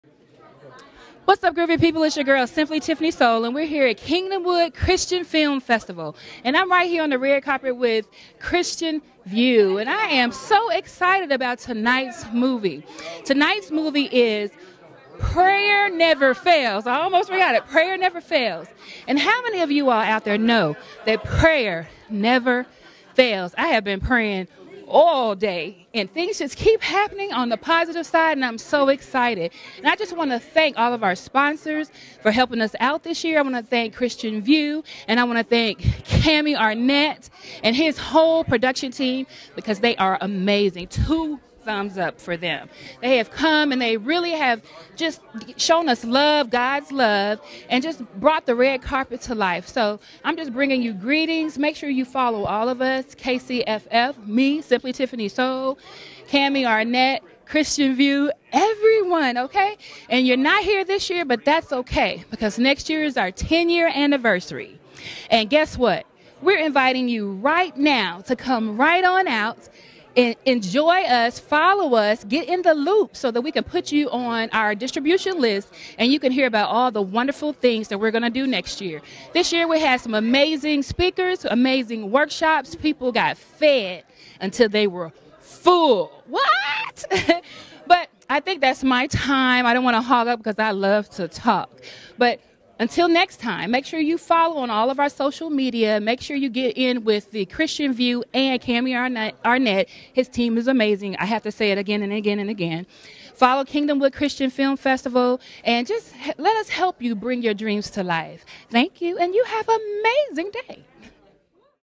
Interview with Tiffany Roberts
CAPS in conjunction with The Christian View at the 2016 Kingdomwood Film Festival.